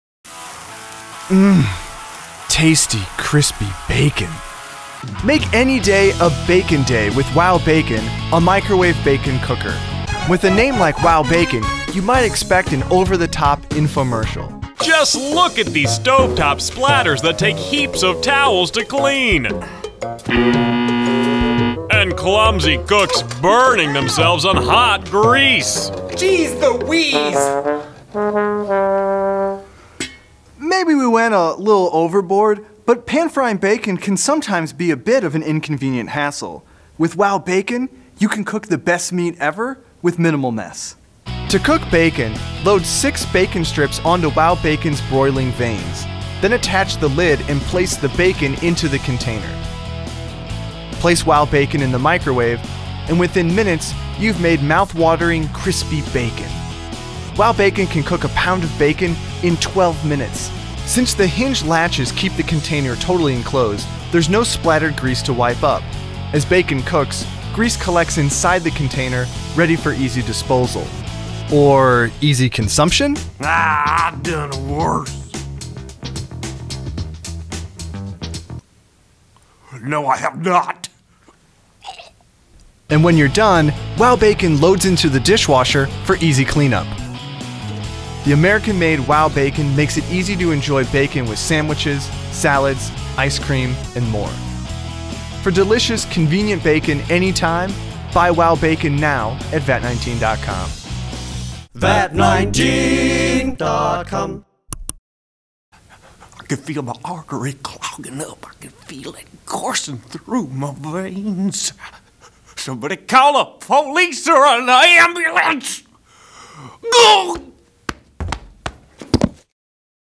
Make Perfect Bacon in the Microwave
Cook crispy bacon in minutes with the WowBacon Microwave Cooker. This ingenious broiler allows you to prepare six strips of crispy bacon without any splatter or mess.

WowBacon's enclosed design ensures all drippings are contained and pooled for easy disposal. No more paper towels!

Made in the USA, the WowBacon Microwave Cooker allows you to make as much bacon as you want, on the spur of the moment, with set-it-and-forget-it ease.